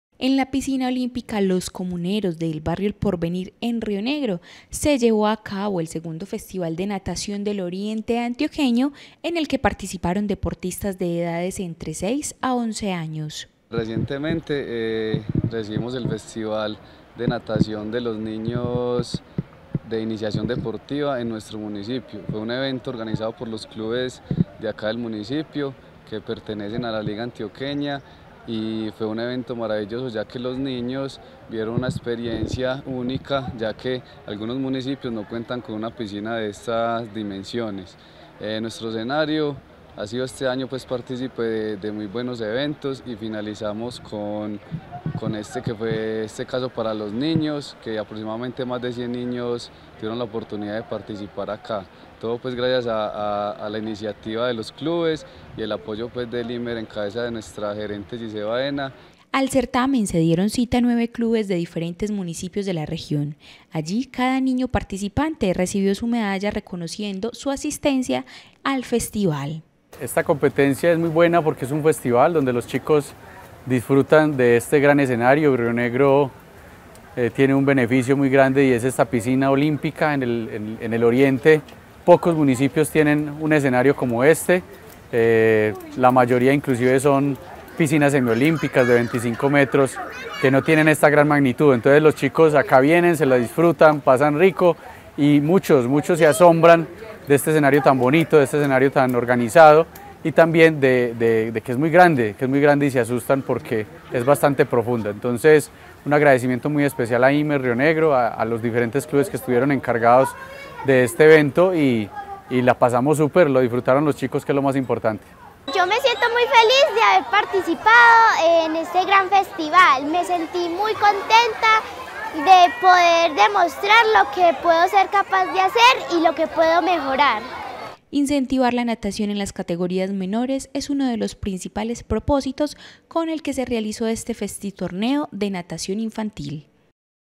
En Rionegro se realizó un Festival de Natación
Incentivar la natación en las categorías menores es uno de los principales propósitos con los que se realizó este festitorneo de natación infantil.